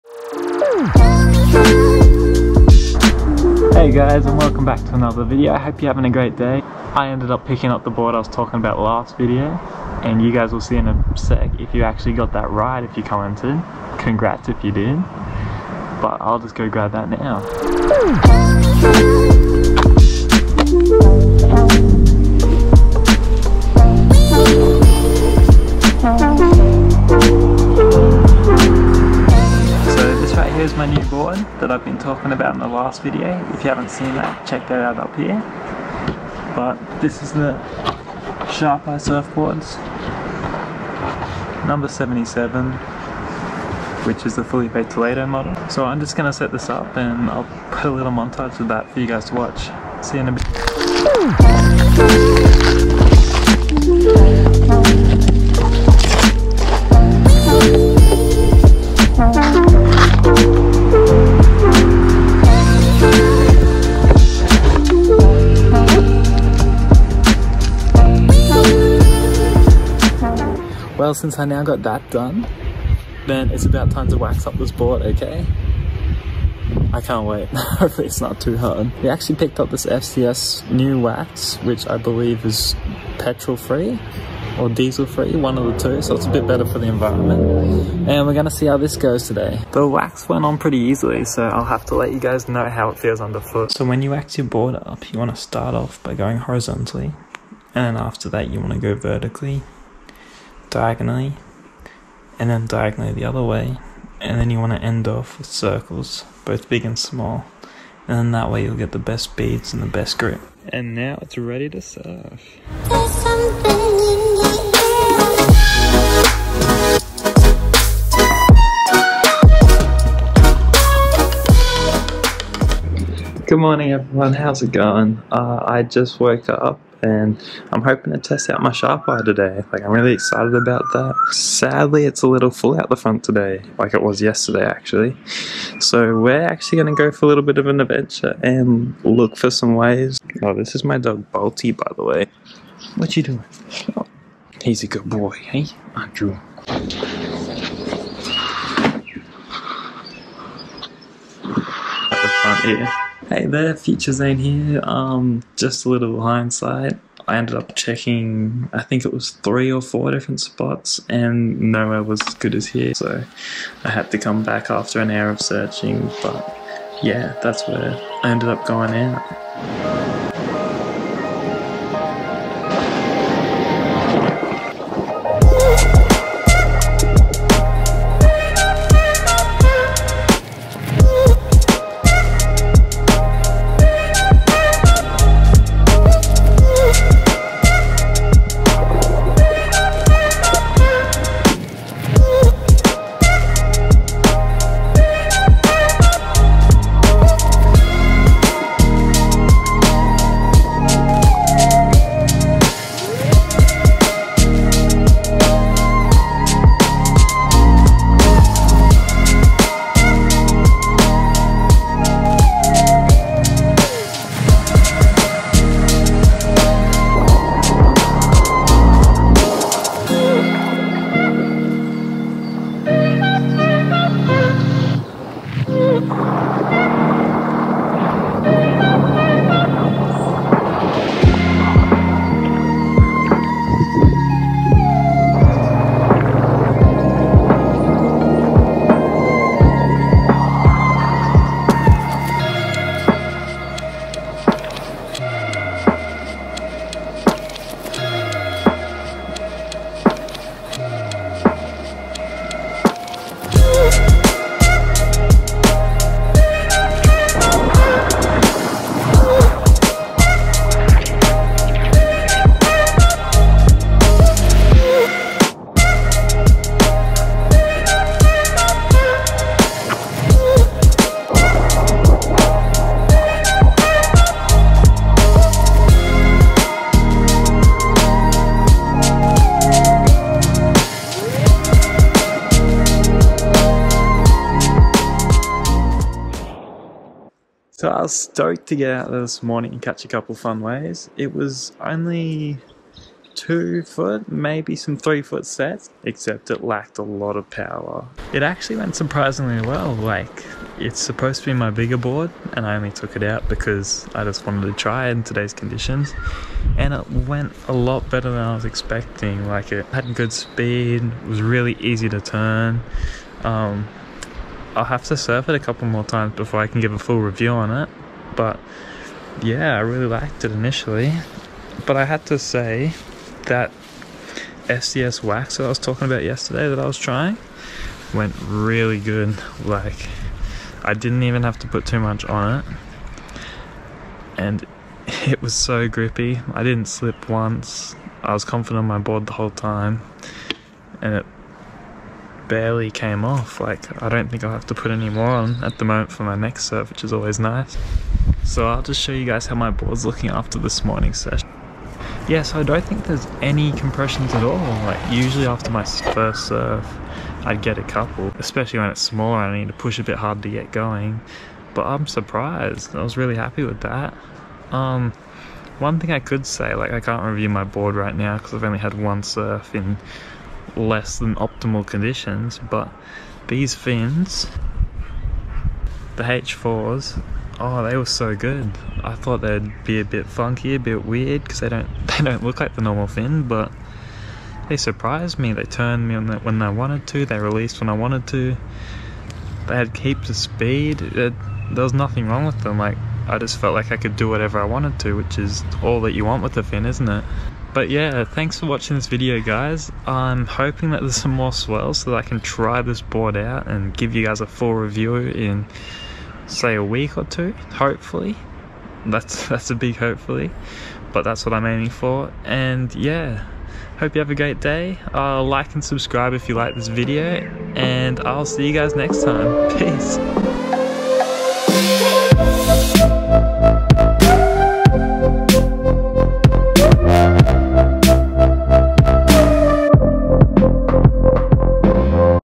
CLEAN Winter Swell Continues! | I Bought a NEW Board!
I take you through preparing my new board and taking it out for a fun surf in glassy, clean, 2ft small surf. The offshore winds are bringing the cold weather just as I thought winter was starting to end. Hope you all are getting good waves.

2:50 Surf Time!

Please comment and give me some feedback on what you want to see more of!

My Board: Sharpeye #77 5'10